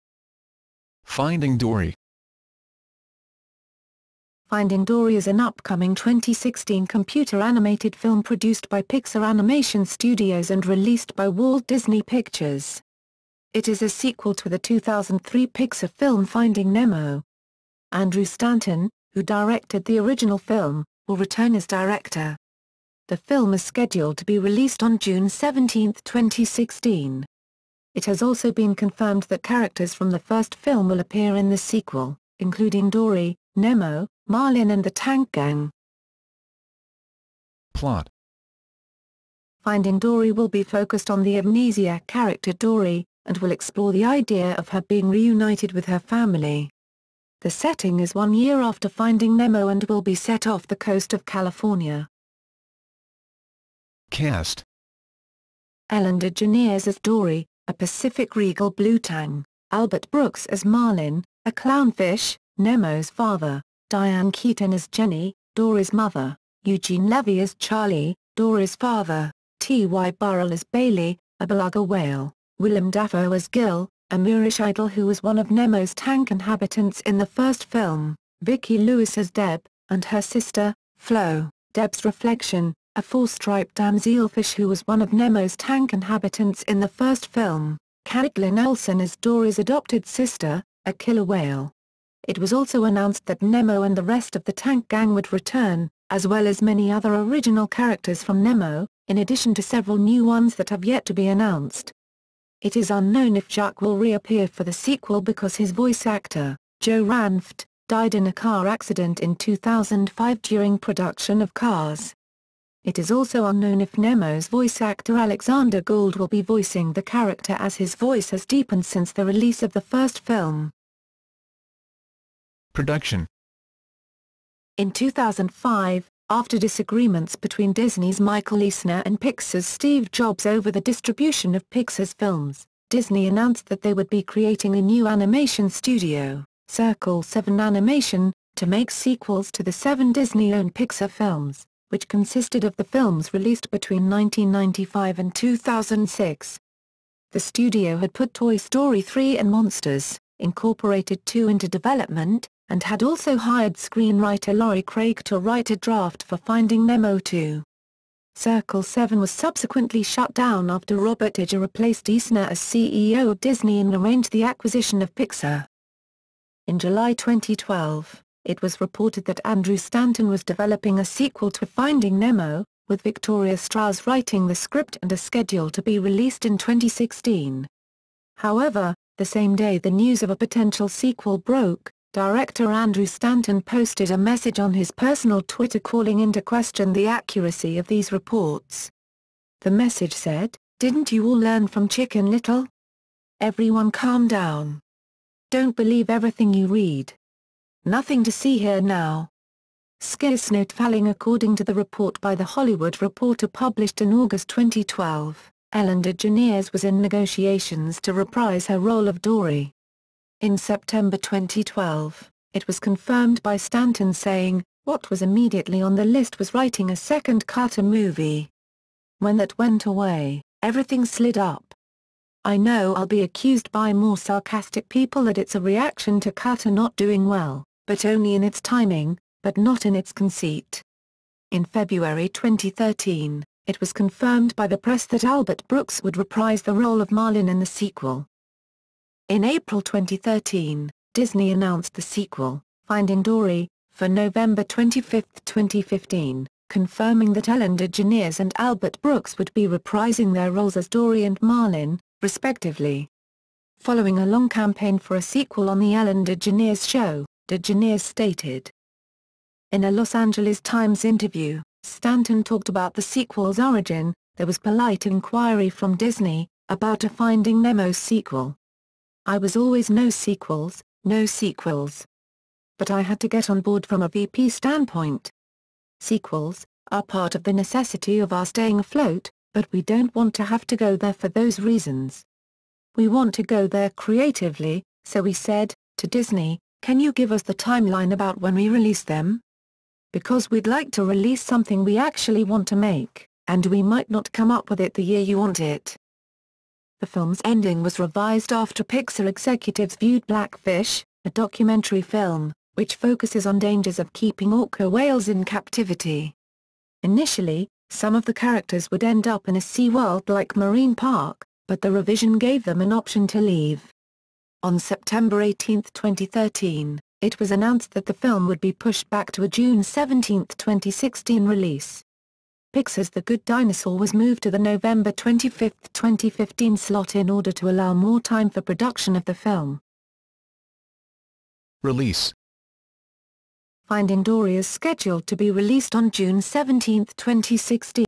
Finding Dory
Finding Dory is an upcoming 2016 computer-animated film produced by Pixar Animation Studios and released by Walt Disney Pictures. It is a sequel to the 2003 Pixar film Finding Nemo. Andrew Stanton, who directed the original film, will return as director. The film is scheduled to be released on June 17, 2016. It has also been confirmed that characters from the first film will appear in the sequel, including Dory, Nemo, Marlin and the "Tank Gang."

Attribution:
Article text available under CC-BY-SA
Public domain image source in video